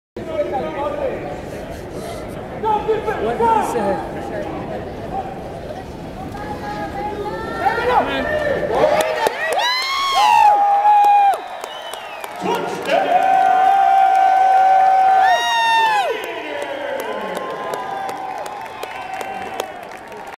First Home Touchdown!
Kasey Peters throws a touchdown to someone...  I don't know which names go with which numbers yet.  :-(